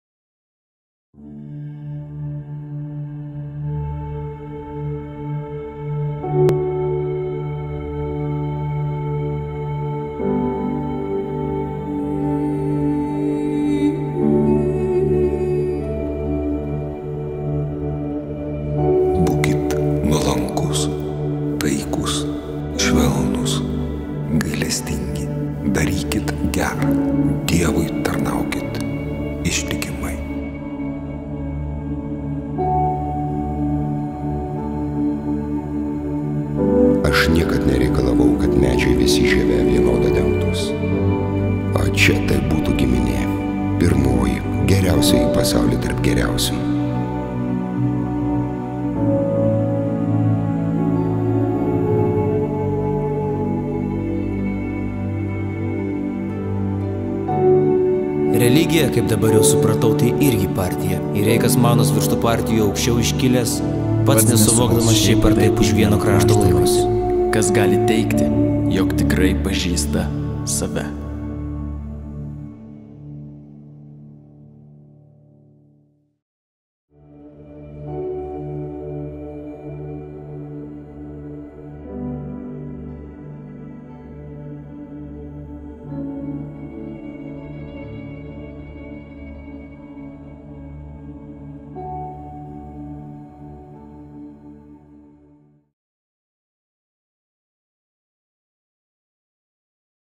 NATANAS IŠMINTINGASIS (rež. G.Varnas)
Gotthold Ephraim Lessing
Natanas Išmintingasis
Dviejų dalių spektaklis

„Visų pirma žmogus – o tik paskui – krikščionis, musulmonas arba žydas“ (Natanas) „Aš niekad nereikalavau, kad medžiai visi žieve vienoda dengtūs.“ (Saladinas) „Skubėk į savo dykumą sugrįžt. Bijau, kad tarp žmonių gali užmiršti, kaip būti žmogumi.“ (Natanas)
Režisierius Gintaras Varnas
Scenografas Gintaras Makarevičius
Kostiumų dailininkas Juozas Statkevičius
 Lėlių dailininkė Julija Skuratova
Kompozitorius Vidmantas Bartulis
Video menininkas Rimas Sakalauskas
 Kūrybinis bendradarbis Antanas Obcarskas
Vaidina Vytautas Anužis (Natanas Išmintingasis ) Dainius Svobonas (Sultonas Saladinas)  Vainius Sodeika (Tamplierius) Arnas Ašmonas (Tamplierius )  Eglė Špokaitė (Sita)  Jovita Jankelaitytė (Recha) Vilija Grigaitytė (Daja )  Saulius Čiučelis (Dervišas Al Hafis) Liubomiras Laucevičius (Jeruzalės patriarchas)  Sigitas Šidlauskas (Vienuolis) Vaidas Maršalka (Žydas, musulmonas, krikščionių vienuolis ) Gintautas Bejeris (Žydas, musulmonas, krikščionių vienuolis ) Edgaras Žemaitis (Žydas, musulmonas, krikščionių vienuolis ) Andrius Gaučas (Žydas, musulmonas, krikščionių vienuolis )